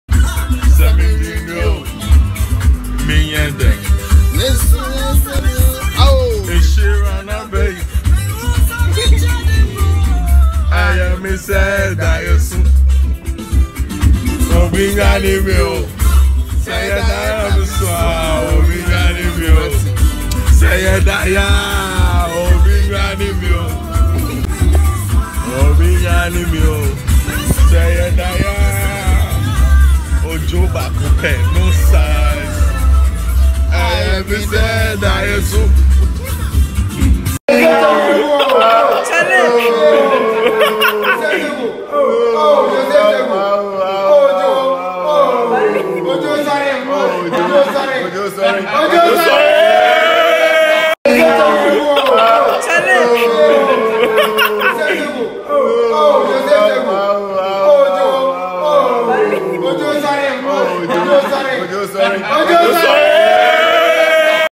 Baby Joe does the”Resurrection Challenge” with Peter Danso Mensah😂
Baby Joe and Peter Danso Mensah having at the Black Meteors (Under 23) Camp